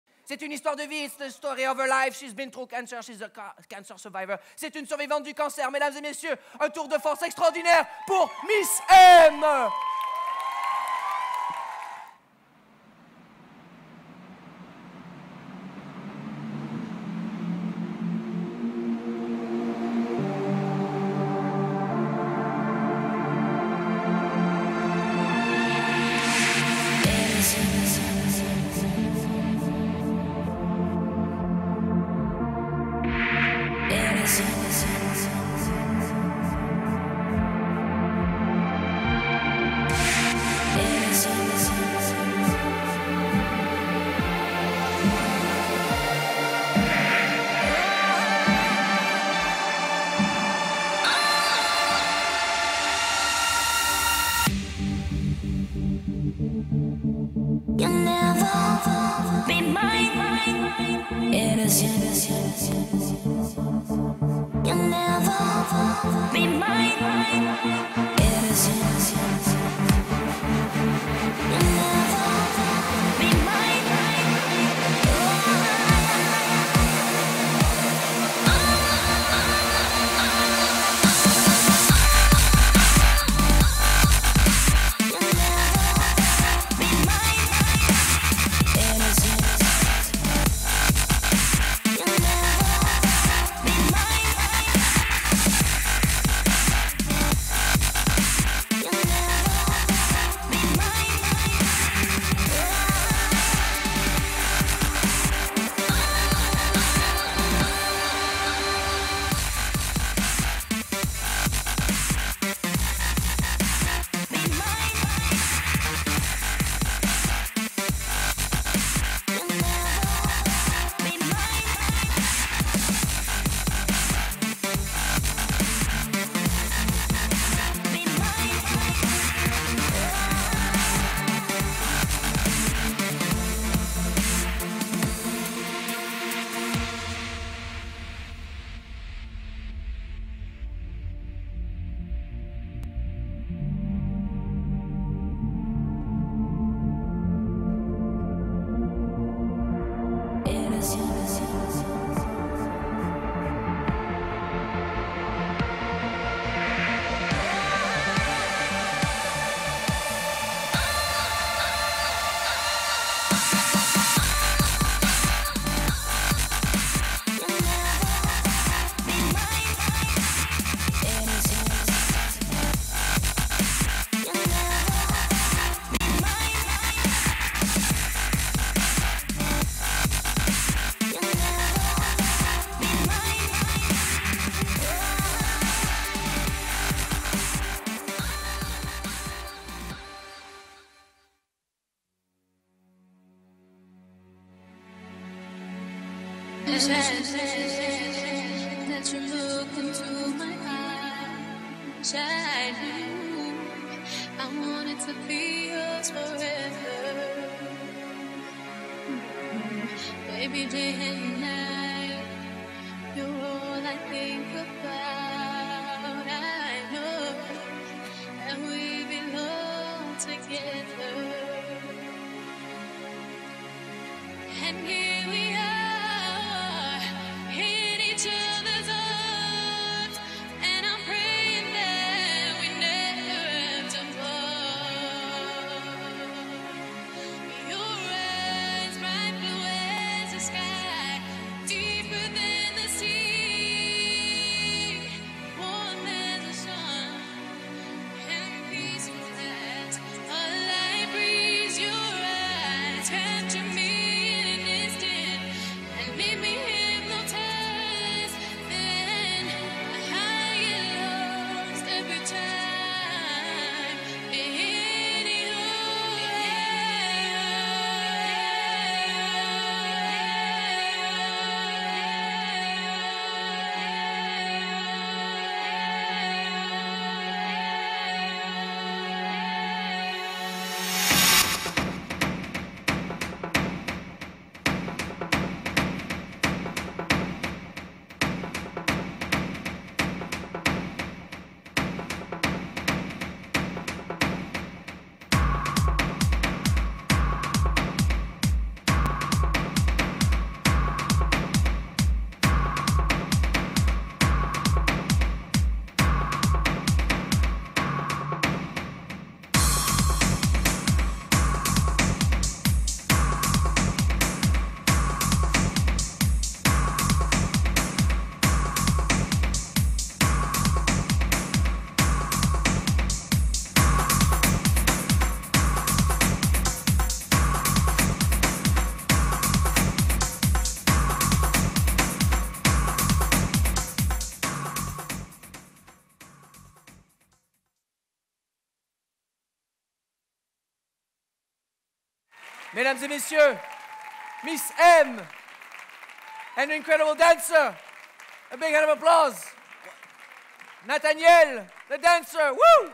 Maya Melata Grand Burlesque Show March 2013
M, known as Maya Melata, performed her first ever Burlesque Show at the Grand Burlesque Show 2013 in Montreal! Her performance was built on her experience of over coming stage 4B cancer (Hodgkin's Lymphoma).